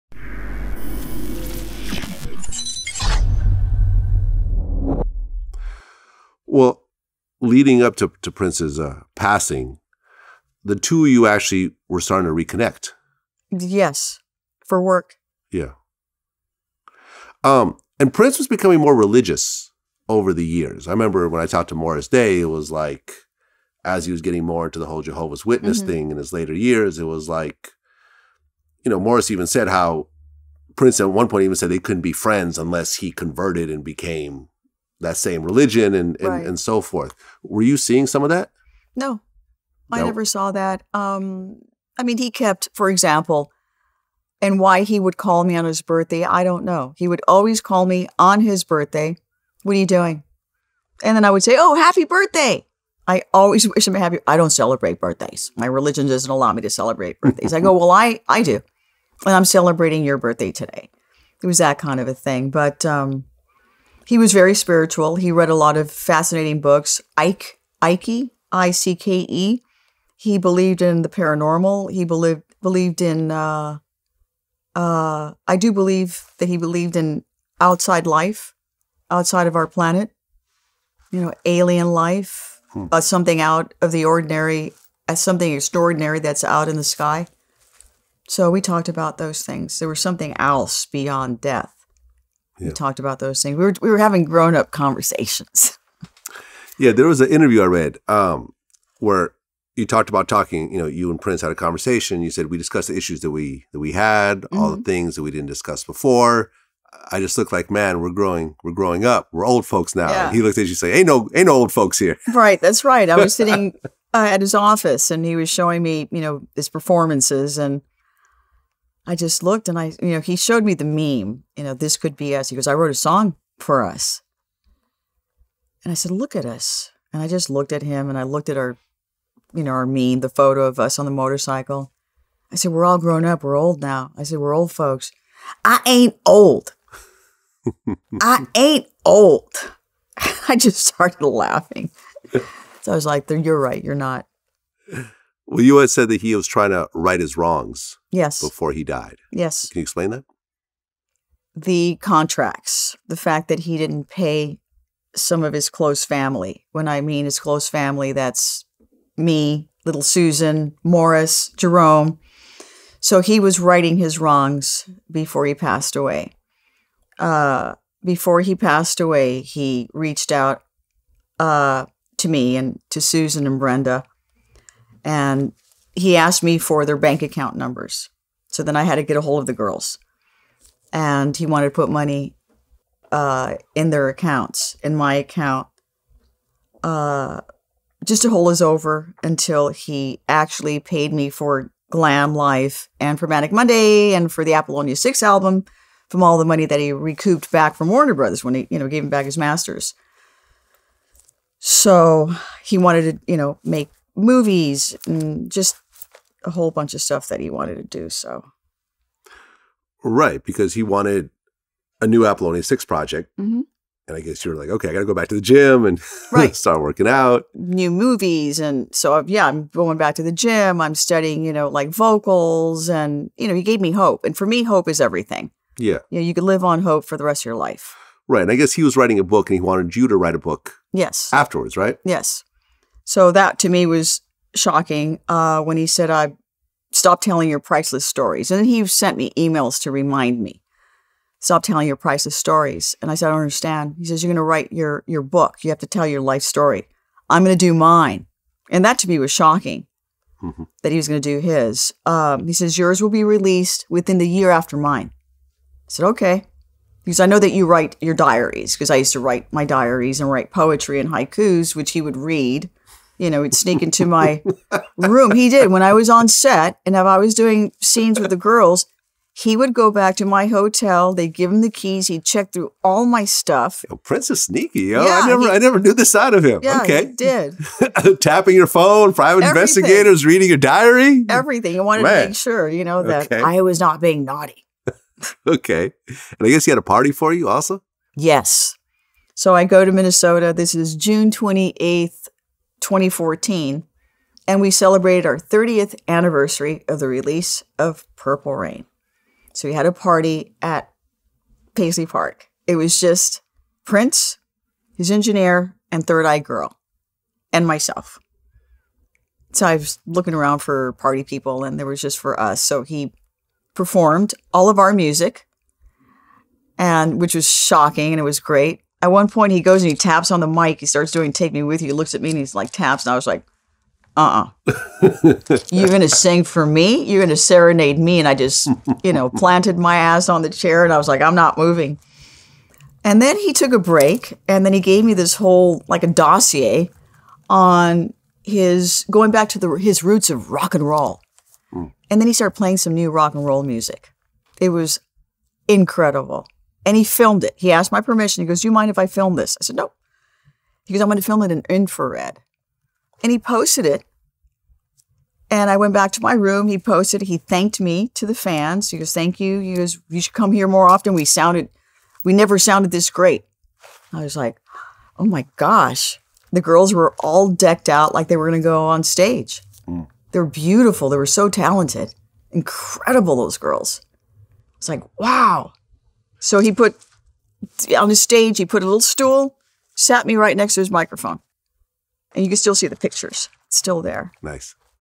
Apollonia: Prince was Trying to Right His Wrongs Before He Died (Part 12)
In this clip, Apollonia reflected on reconnecting with Prince after decades and how much they had grown over that time in addition to how much their dynamic evolved. She recounted some of the sneaky things Prince would do to check on her, detailed a concert he gave her for her birthday, and how he tried to "right his wrongs" with close family before passing away.